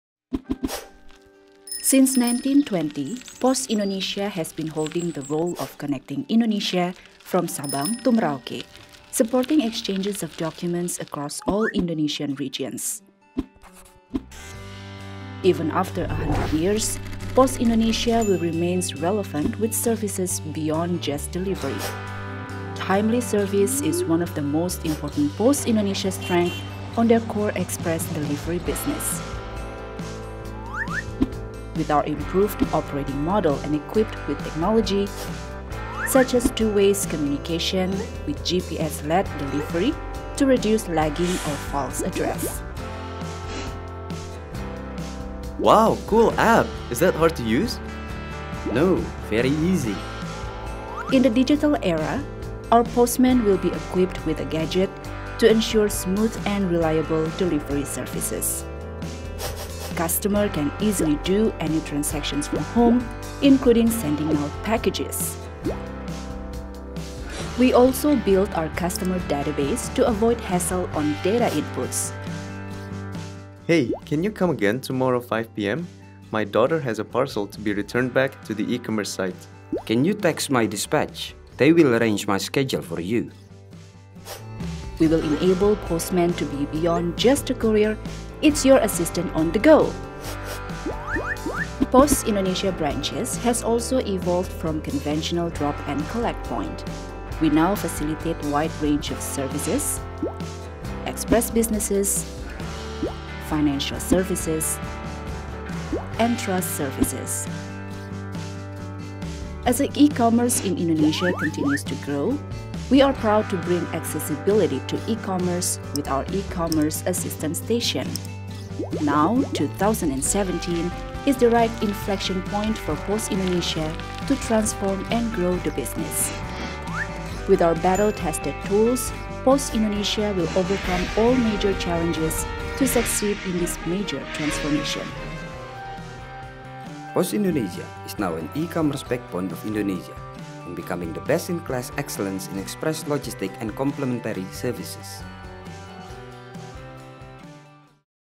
Pos Indonesia x BCG
a motion graphic animation about history of Pos Indonesia and its relation with Boston Consultant Group